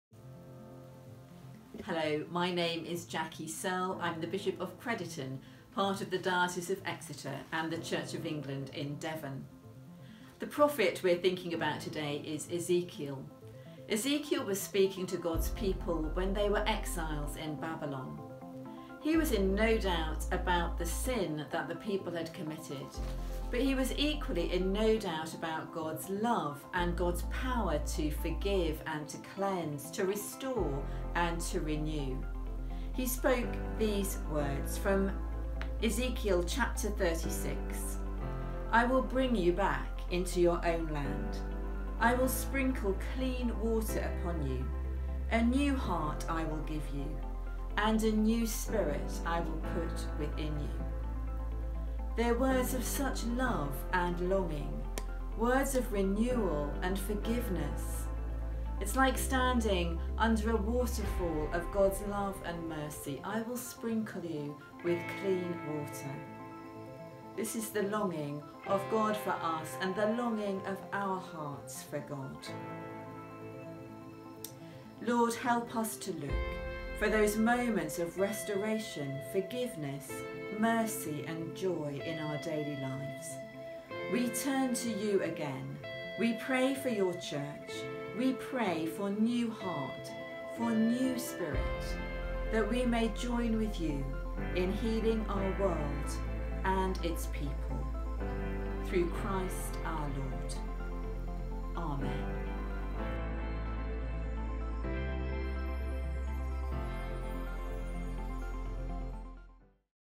DAY 3 | Jackie Searle, Bishop of Crediton
He shows us love and mercy! 🙏 Today we look at Ezekiel 36:22,25.

Each day church leaders from across Exeter share inspirational thoughts and prayers as we set our eyes on Jesus and hold onto His promises for us.